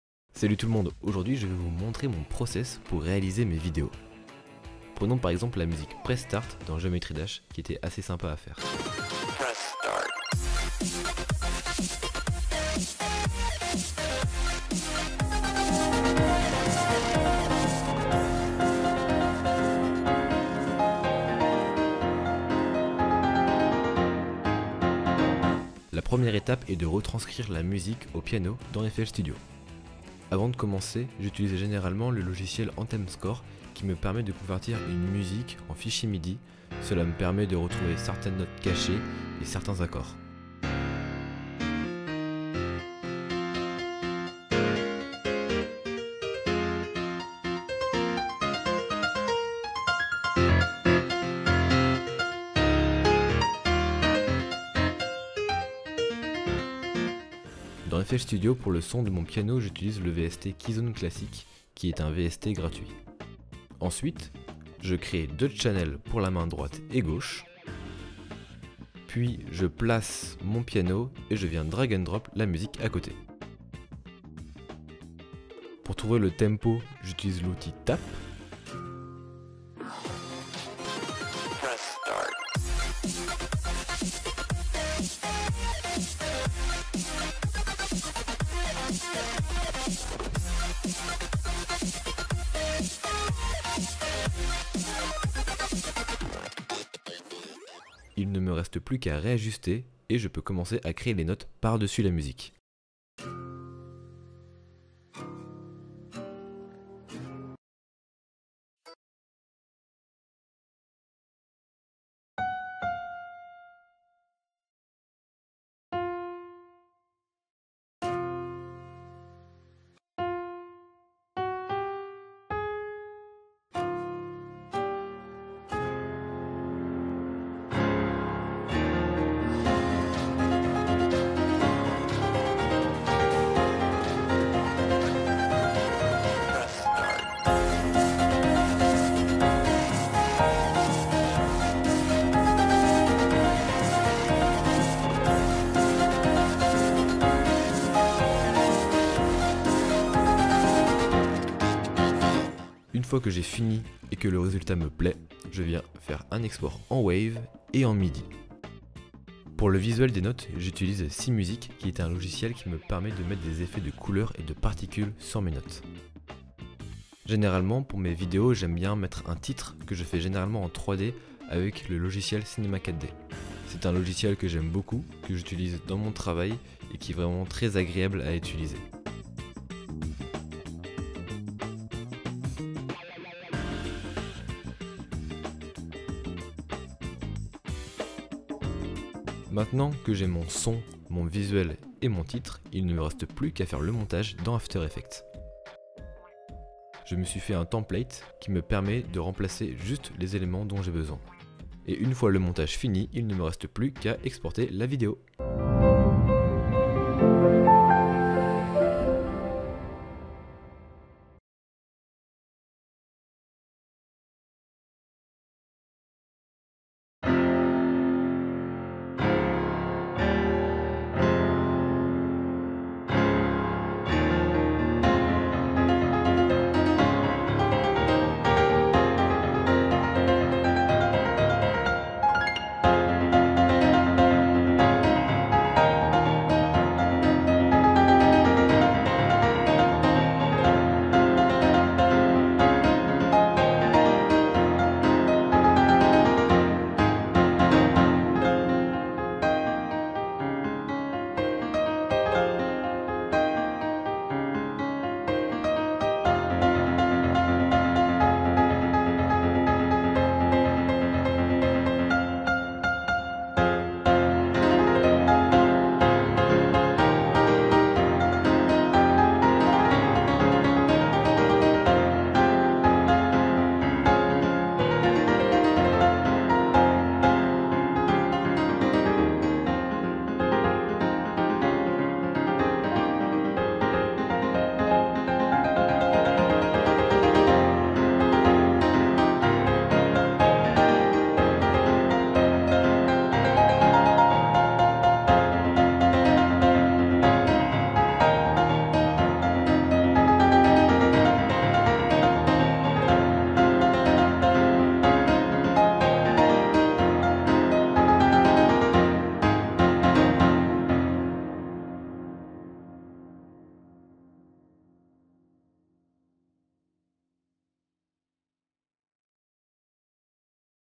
HOW TO MAKE PIANO VIDEOS?
✨ Hi everyone !! ✨ Today, new video ! HOW TO MAKE PIANO VIDEOS?😊
Take care ❤️

Softwares used :
- 🎵 FL Studio
- 🎼 AnthemScore
- 🎹 SeeMusic Visualizer
- ❤️ Cinema4D
- 🎬 After Effect
- 🖼️ Photoshop